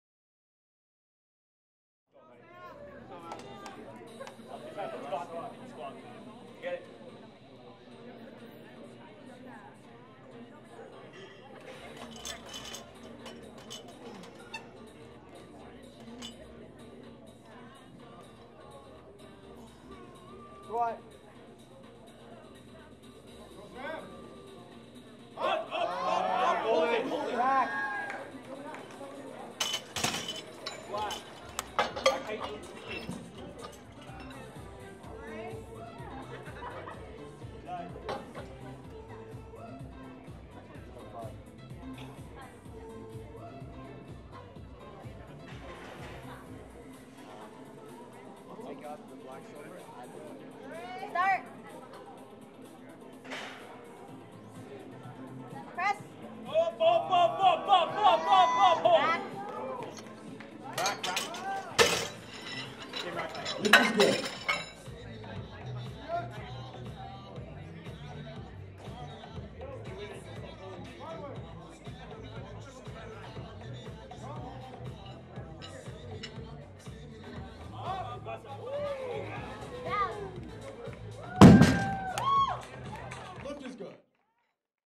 Samuel Pereira - Seneca College Powerlifting Meet - Mar.25.2023
Highlights of Redeemer's Samuel Pereira at the 2023 Seneca College Powerlifting Meet. The competition took place at Seneca's Markham Campus on March 25, 2023.